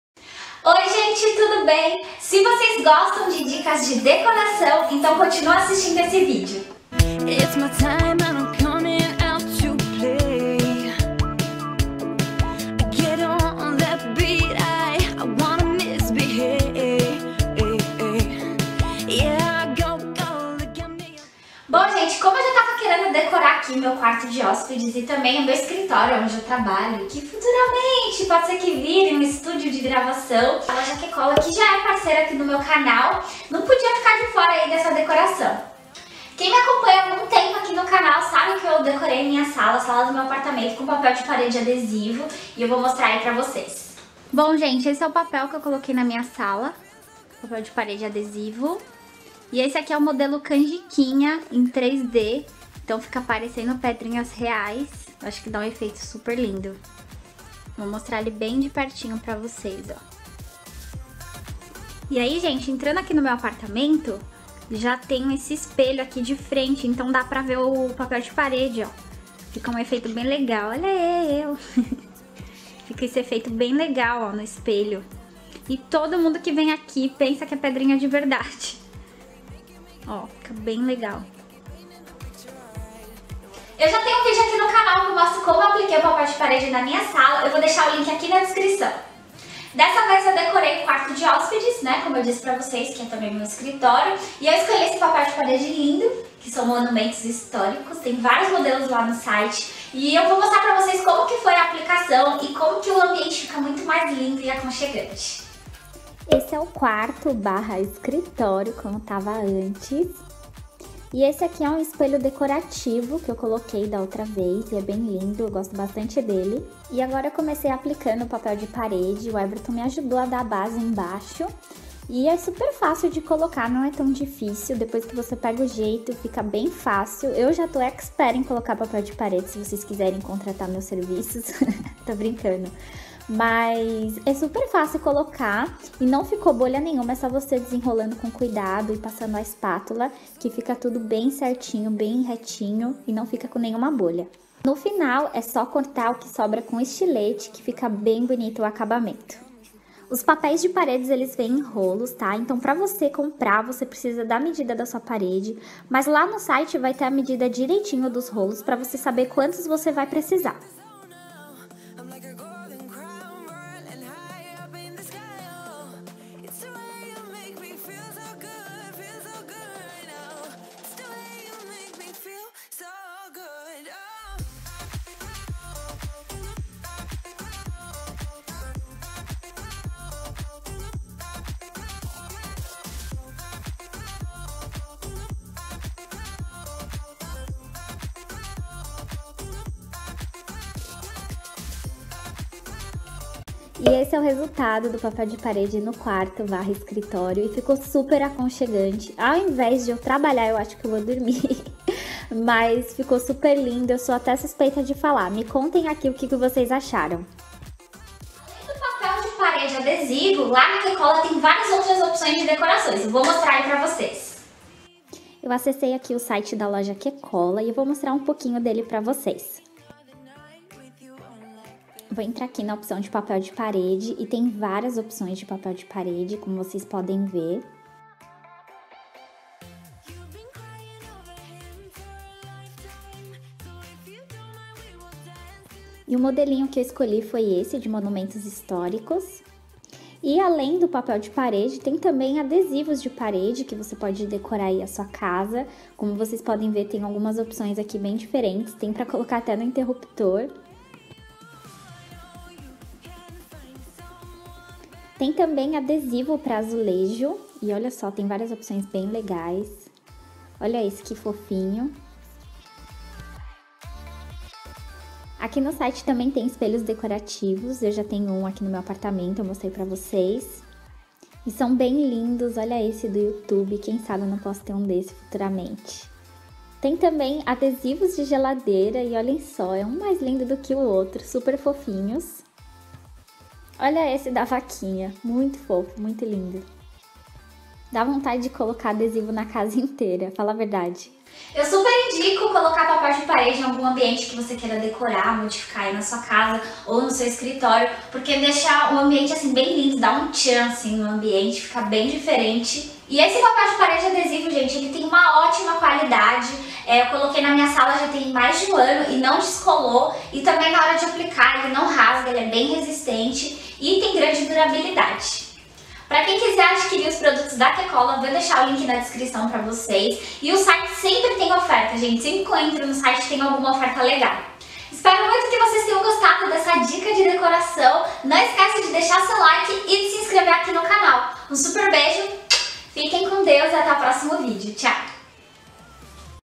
♡ DIY: Decorando o quarto/escritório com Papel de Parede Adesivo - Qcola ♡
Oii gente, tudo bem?

Nesse vídeo mostrei pra vocês como decorei o quarto de hóspedes/escritório com papel de parede adesivo.

Face: /qcola

Beijos.